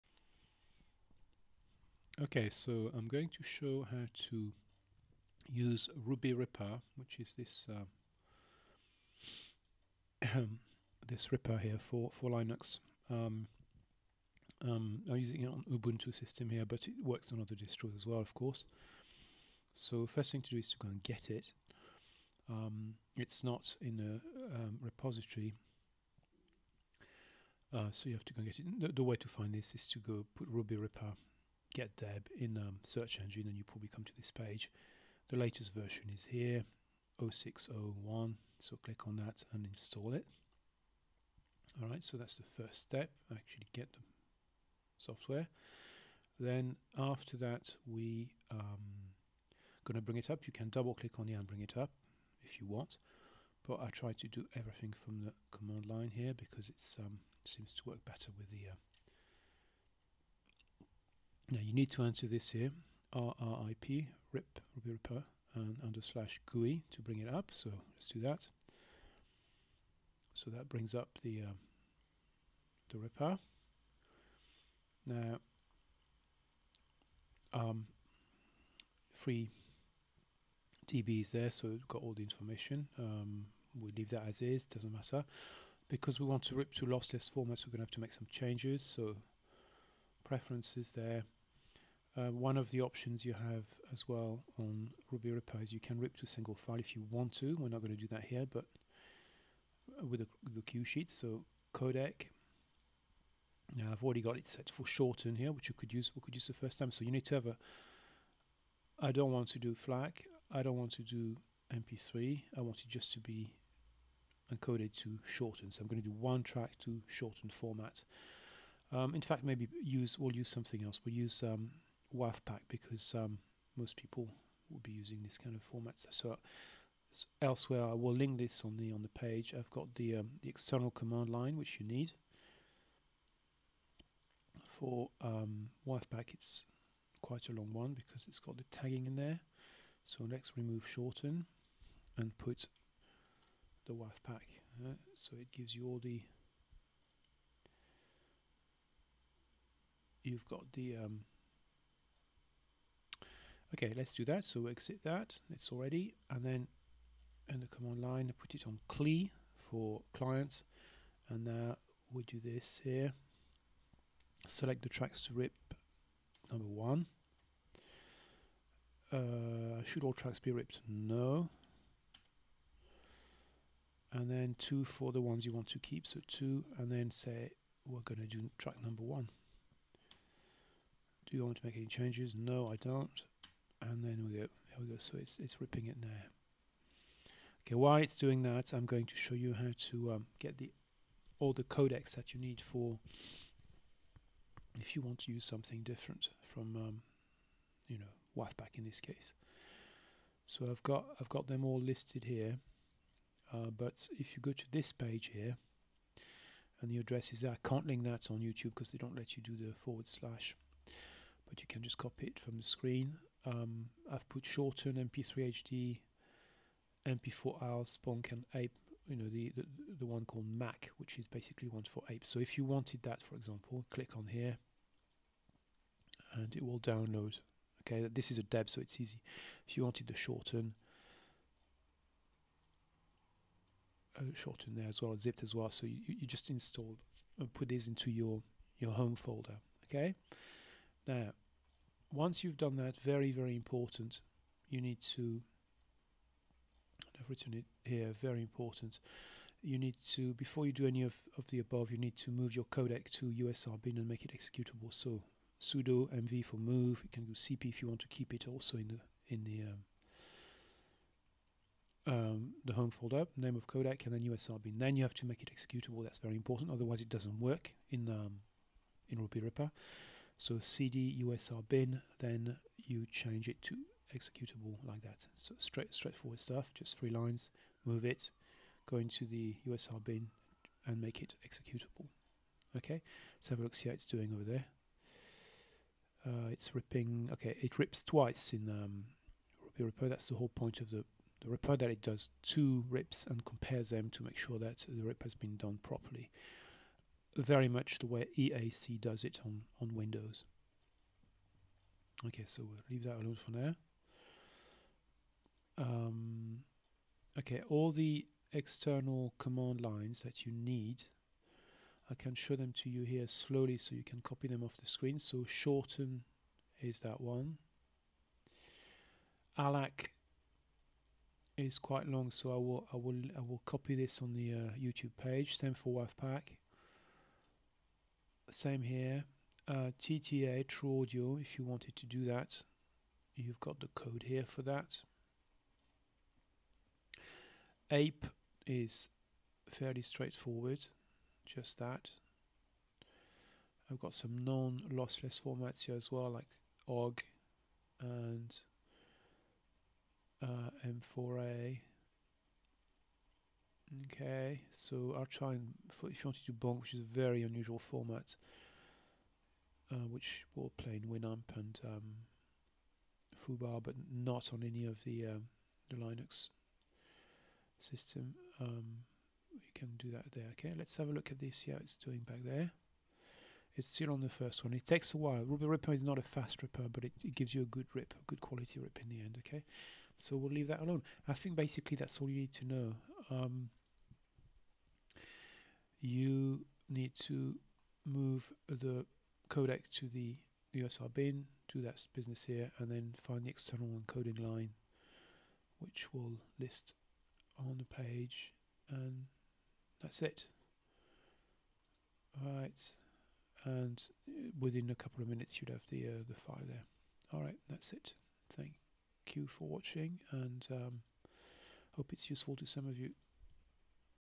How to rip to lossless on Ubuntu using Rubyripper
How to rip to lossless using Rubyripper. For Linux/ubuntu and other distros

alac

ffmpeg -i %i -acodec alac "%o".m4a && neroAacTag %o.m4a -meta:artist=%a -meta:album=%b -meta:genre=%g -meta:year=%y -meta:track="%n" -meta:title="%t"

wavpack

wavpack -w "Artist=%a" -w "Title=%t" -w "Album=%b" -w "Year=%y" -w "Track=%n" -w "Genre=%g" -hhm %i -o "%o.wv"

ape with tags id3v2

mac "%i" "%o".ape -c5000 && id3v2 %o.ape --artist %a --album %b --song %t --genre %g --track %n

mac "%i" "%o".ape -c5000 && apetag -i %o.ape -m overwrite -p trackname=%t -p Artist=%a -p Album=%b -p Year=%y -p Track=%n -p Codec=%f

tta

ttaenc -e "%i" -o "%o".tta && id3v2 %o.tta --artist %a --album %b --song %t --genre %g --track %n --year %y

my favourite

shorten also can be tagged with id3

shorten "%i" "%o".shn && id3v2 %o.shn --artist %a --album %b --song %t --genre %g --track %n --year %y --comment Ripped.with.Rubyripper